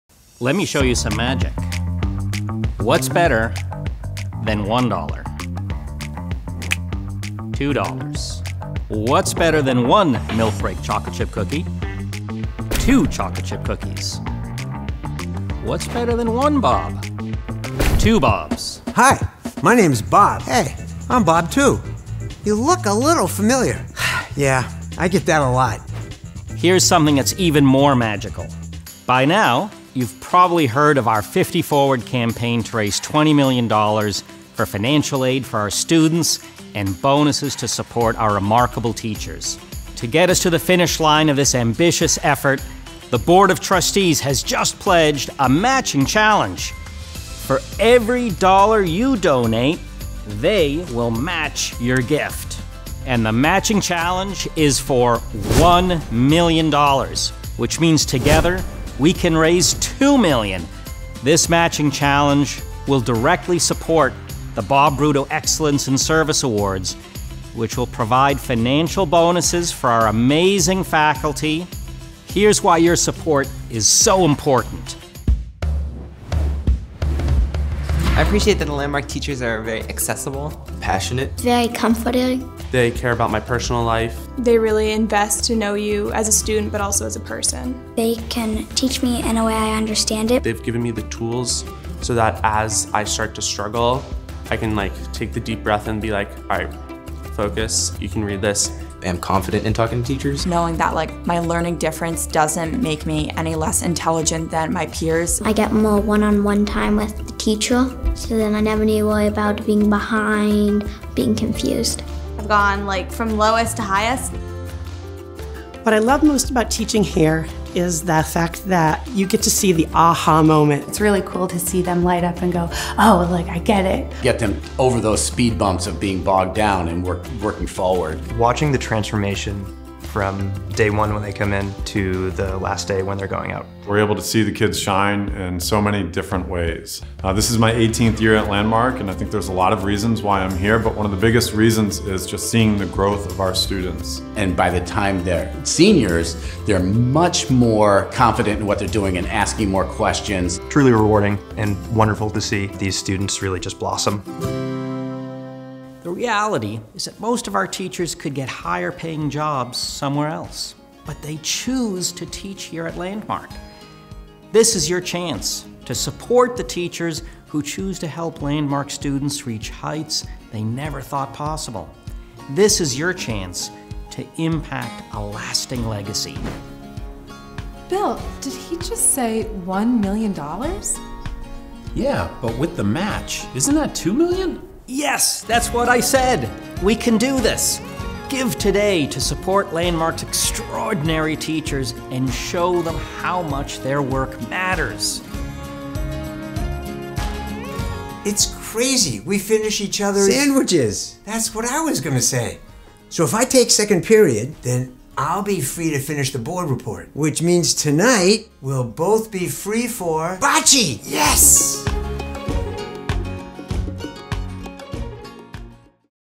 What's Better Than $1 Million for Landmark Teachers?
$2 Million for Our Teachers!

Take a look as Landmark's Chair of our Board of Trustees shows you a little magic, and explains how we are making a push to help our teachers keep doing their amazing work changing the lives of students.

Landmark’s Board of Trustees will match all 50 Forward Capital Campaign endowment gifts, dollar for dollar up to $1,000,000, received now through June 30, 2022!

Funds raised will support the Bob Broudo Excellence in Service Awards, to provide monetary bonuses to our amazing teachers at milestone anniversaries.

Give today in support of Landmark's extraordinary teachers to double your gift—and show them how much their work matters!